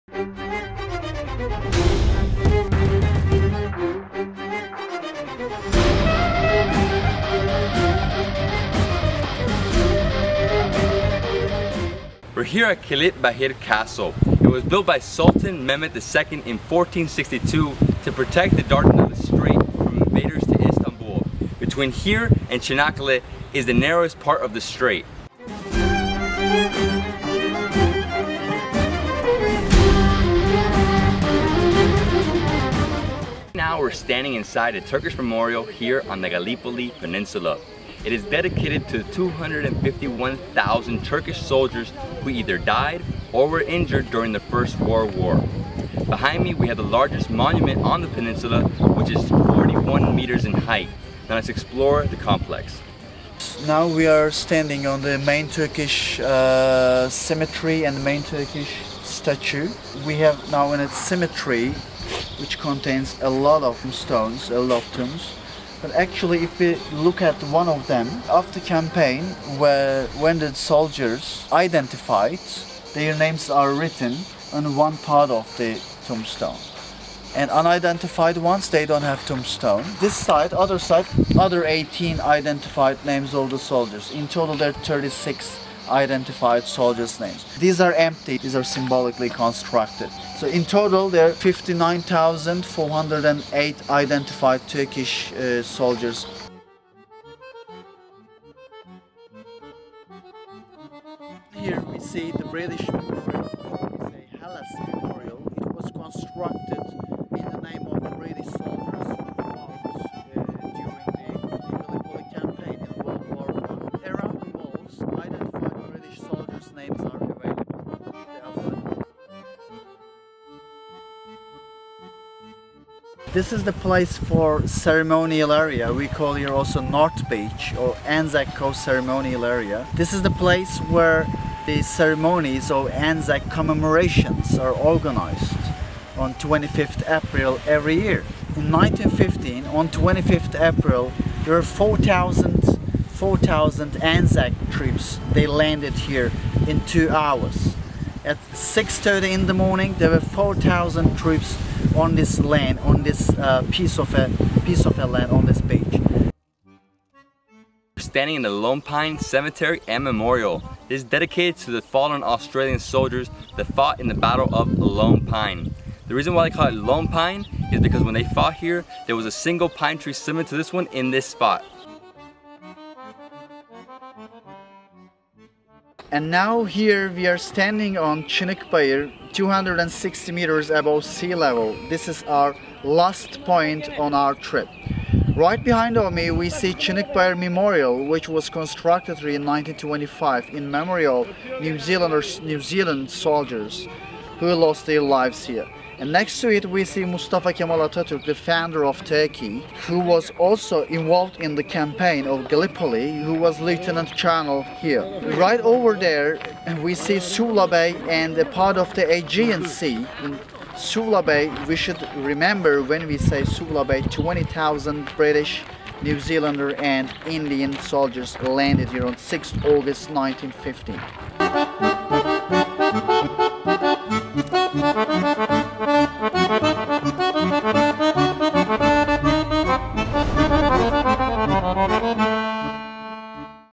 Touring the Gallipoli Peninsula, Turkey
In this episode, David takes us around the main sights of Gallipoli, Turkey - a peninsula in the Dardanelles Strait that has long been known for its strategic position on the main route between Europe and Asia. In other words, whoever or whatever wanted to pass through the Marmara Sea into Constantinople (present-day Istanbul) would first have to cross the Dardanelles Strait. Thus, Gallipoli has always been at the center of military action throughout the centuries.

On April 25th, 1915, the British tried to capture Constantinople by launching an attack on Gallipoli. This is known as the Gallipoli Campaign. Today, visiting Gallipoli reveals just how important the peninsula was for Allied forces to possess in WWI. Touring the Gallipoli Peninsula shows just how many casualties there were from around the world.

David starts his tour on the west side of the peninsula, at the 15th century Kilitbahir Castle, to see exactly how the Ottomans defended the Dardanelles Strait. The location of Kilitbahir Castle is a strategic one - it is positioned at the narrowest part of the strait.

Next up, David shows us the impressive Martyr’s Memorial to honor the 250,000+ Ottoman troops that perished in WWI. The memorial features a massive rectangular monument, sculptures and a expansive cemetery of symbolic tombstones for the Ottoman soldiers who died in the war. Each tombstone has the named of 36 soldiers.

Ottoman soldiers are not the only ones who died during the Gallipoli Campaign. David’s guide Emre takes David to see the Hellas Memorial dedicated to the British soldiers. Their names are inscribed on the sides of the monument’s square platform. Emre and David then go to the Anzac Cove Ceremonial Area, a beach where a memorial ceremony is held each year on April 25th to commemorate the Gallipoli Campaign. Anzac Coce was where Australian and New Zealand troops invaded Gallipoli.

Lone Pine Cemetery and Memorial is another war memorial dedicated to the Australian soldiers who died in the Battle of Lone Pine in August 1925.

The last stop on Emre and David’s tour of Gallipoli takes them to Chunuk Bair Memorial dedicated to New Zealand soldiers.

☆ Equipment Used ☆
↠ Main Camera
↠ Main Lens
↠ Second Lens

Production by Carlos de Varona from Chromahouse.

SEND ME STUFF:
USA